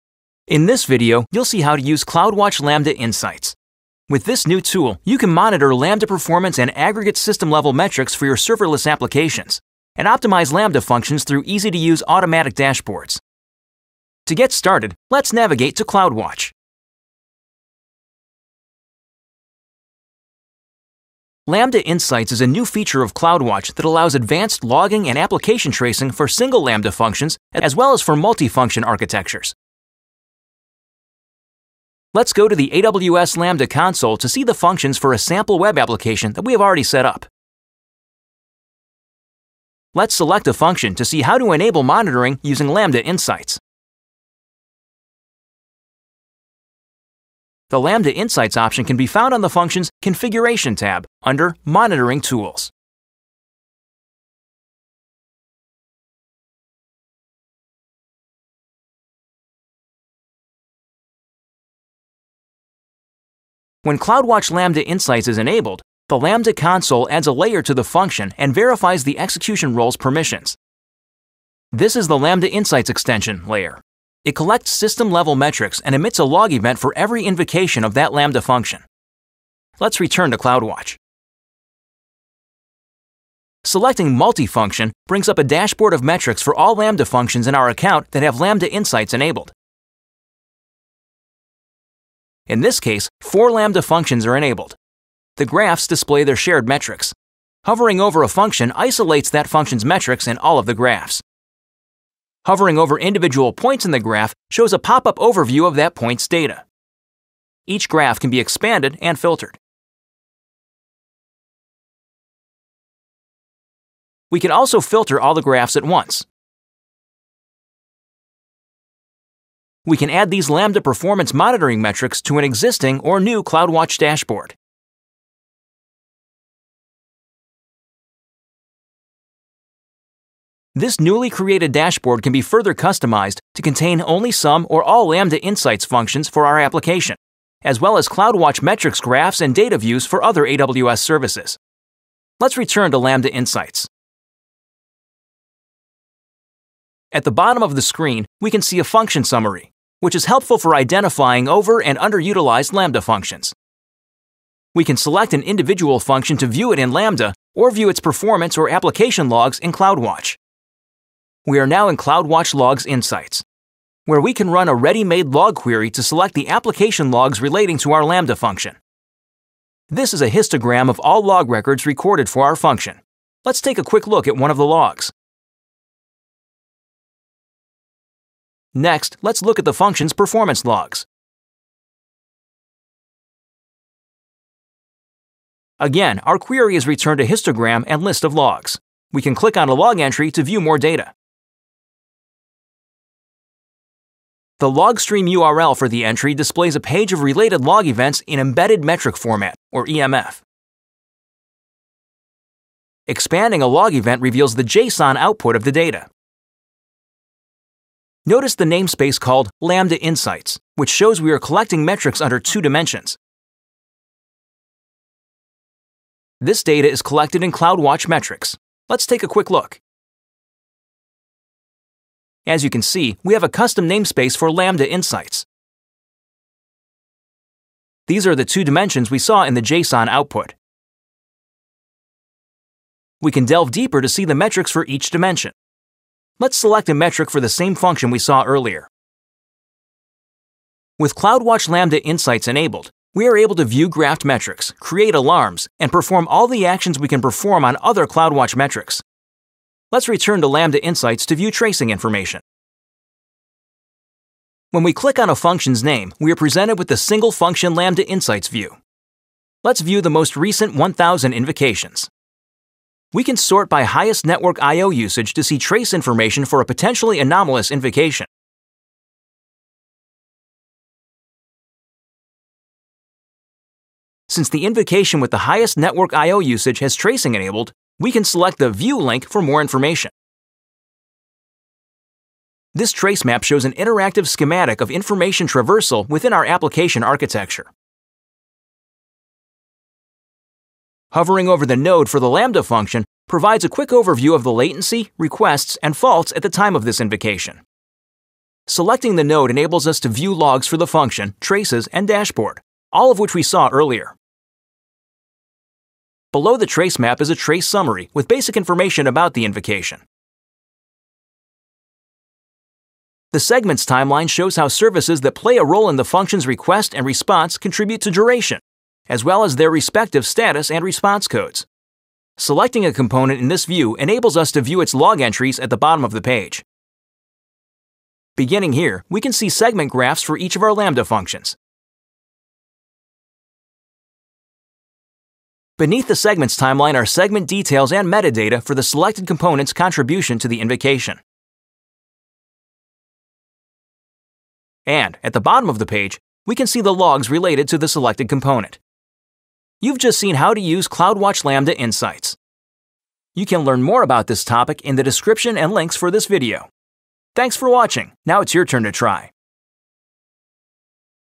Using CloudWatch Lambda Insights
In this video, you’ll see how to use CloudWatch Lambda Insights. With this new tool, you can monitor Lambda performance and aggregate system-level metrics for your serverless applications, and optimize Lambda functions through easy-to-use automatic dashboards.